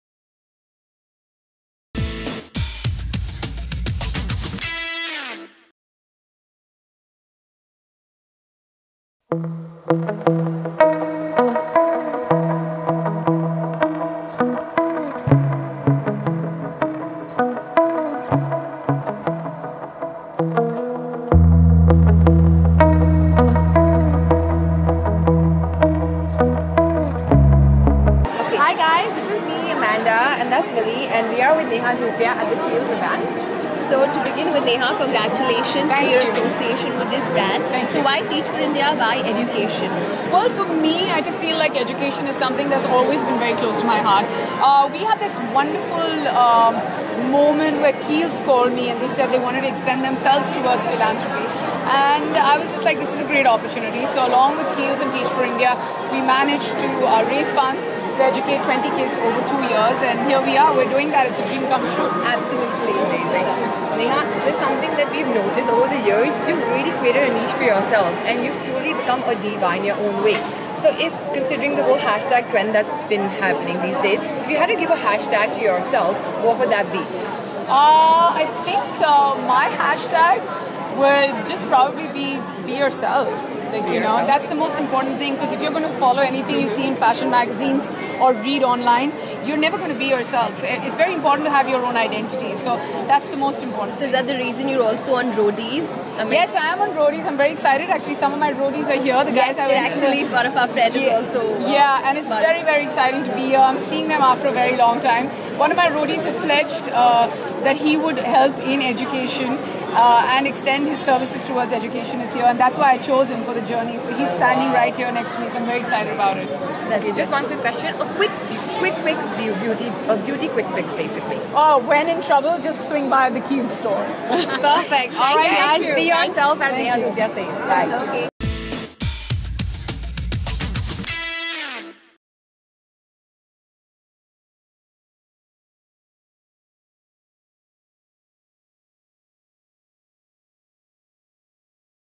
Neha Dhupia in conversation with Style Coquettes
Bollywood actress Neha Dhupia talks about Kiehl's latest initiative with Teach for India to support education in India.